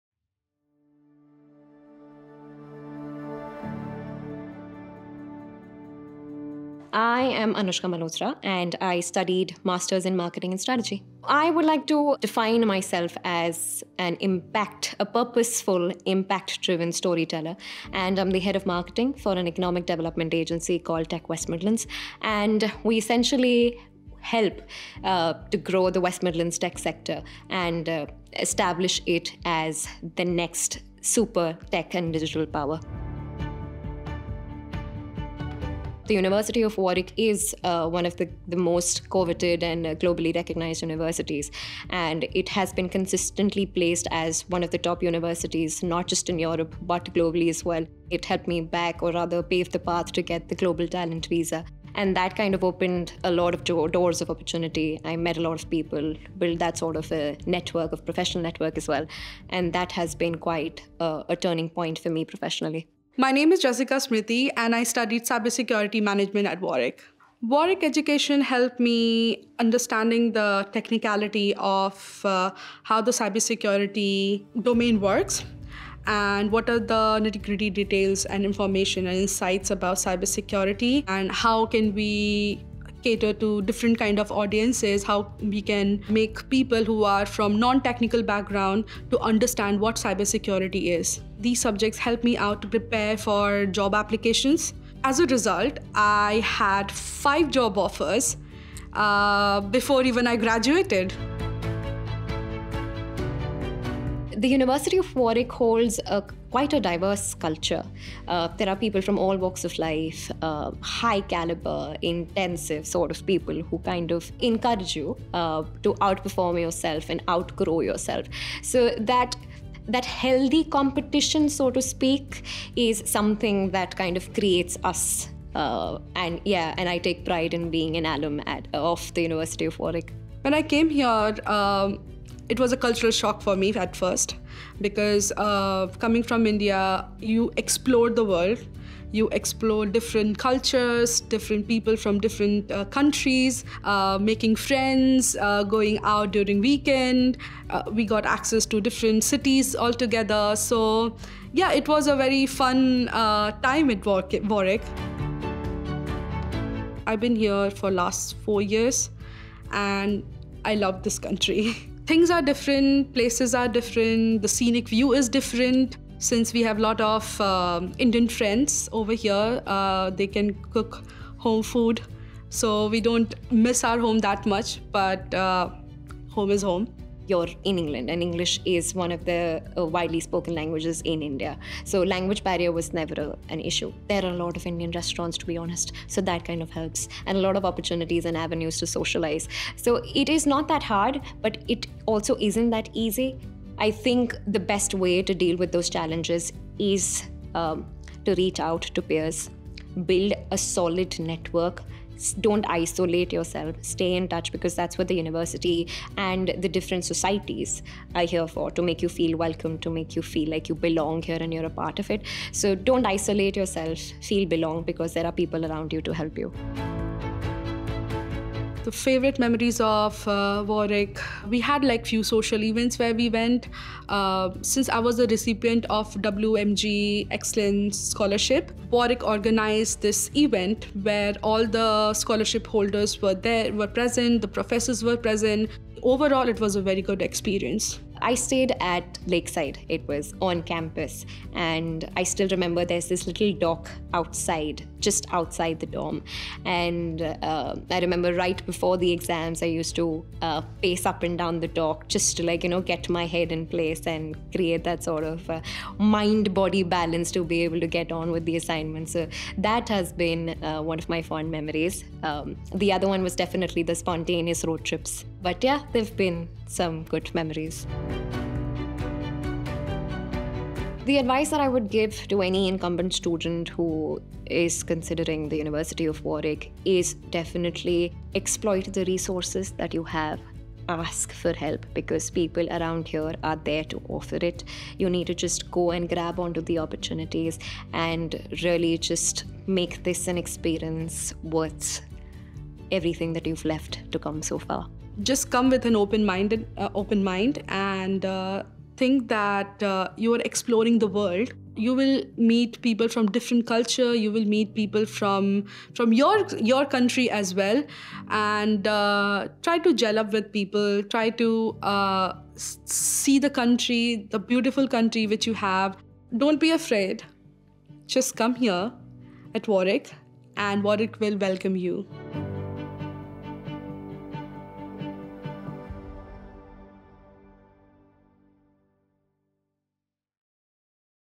University of Warwick Alumni Stories | Offer Holder Day Highlights (Impactful Stories)
Get inspired by the journeys of University of Warwick alumni in this video, crafted for the Offer Holder Day in India on May 24th. This video showcases the experiences and successes of three Warwick graduates, filmed on location at the University of Warwick.

🔹 About This Video:
In collaboration with the University of Warwick, Slate and Mortar produced a compelling alumni story video. This project involved filming interviews with three alumni, capturing their unique experiences and achievements, and editing them into a cohesive, branded narrative.

Our production process involved collaborating closely with the client to create a branded video that would captivate and inspire viewers. We began by conducting interviews on campus with three alumni, delving deep into their stories and successes. These interviews were edited to craft a single, engaging video that would seamlessly weave together their narratives into an inspiring storyline.

👩‍🎓 Featured Alumni:
Hear from graduates about their personal and professional growth.
Gain insights into the impact of a University of Warwick education.
Discover the diverse career paths and achievements of Warwick alumni.

📍 Why the University of Warwick?
This video highlights not only the alumni experiences but also the vibrant campus life and the academic excellence that the University of Warwick offers, making it an ideal destination for prospective students.

Have any questions or need a video like this for your institution?